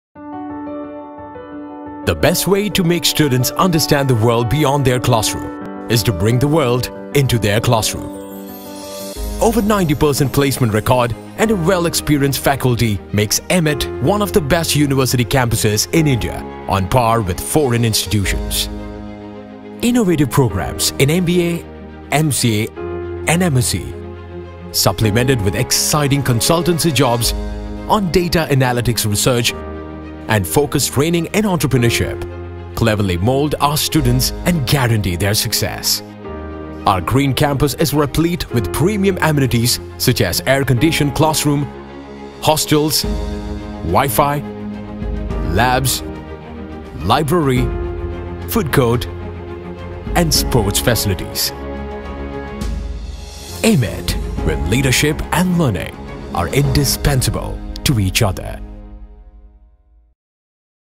AIMIT. St Aloysius College Mangalore.
St Aloysius Institute of Management & Information Technology (AIMIT) is the new campus for MBA and IT programmes offered by St Aloysius College (Autonomous), Mangalore.
The AIMIT MBA & IT Programmes gives you the skills and expertise that will be valued in any industry. We are committed to honing your talents that will help you succeed in a rewarding career.